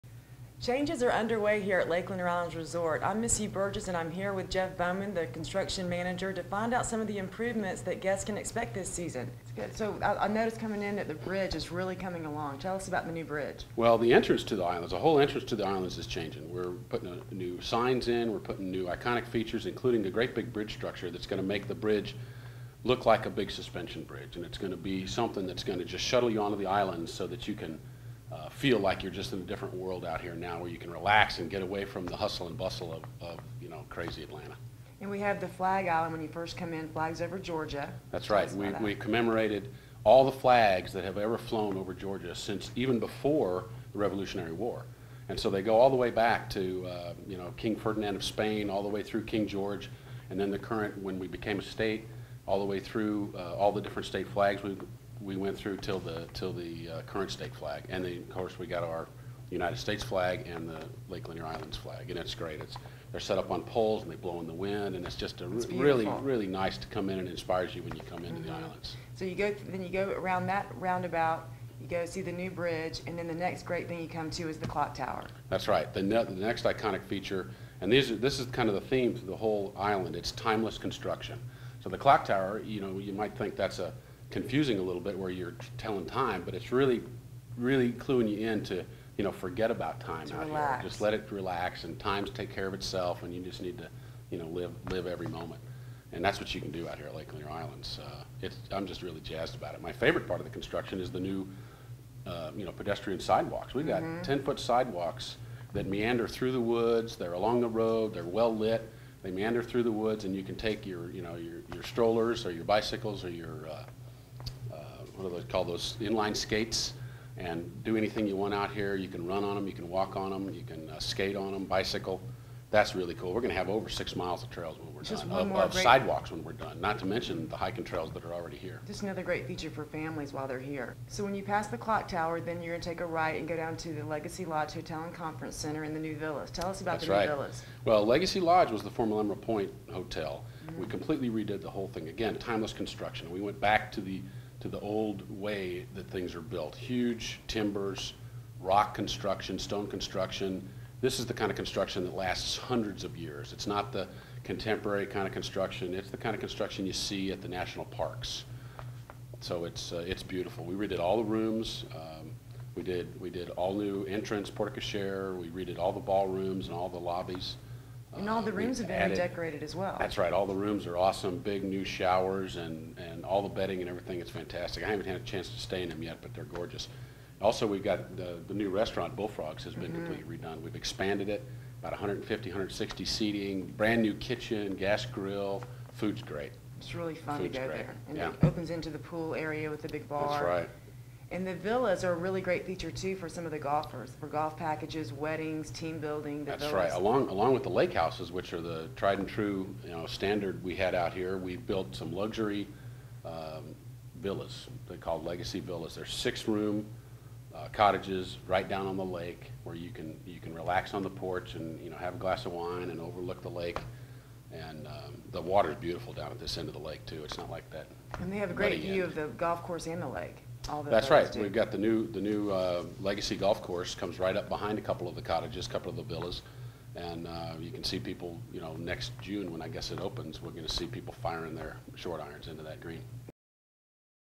An update to Lake Lanier Islands new features
Missy interviews Jeff to discuss the  newly completed components to Lake Lanier Islands infrastructure. They also discussed the hotel now called Legacy Lodge  and discussed the new restaurant called Bullfrogs. They showed some of the first images of the new villas As well.